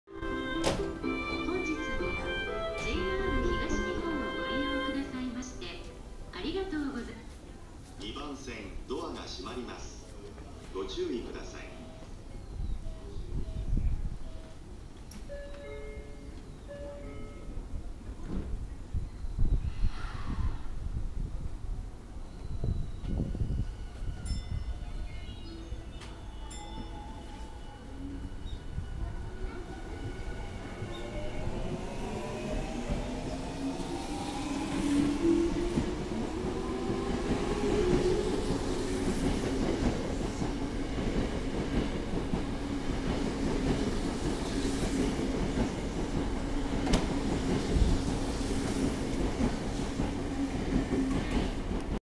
地下鉄東西線（中央・総武線各駅停車）05系各駅停車西船橋行を撮った。三鷹駅
三鷹駅にて
I took this video of the Tozai Subway Line (Chuo/Sobu Line local trains) 05 series local train bound for Nishi-Funabashi. Mitaka Station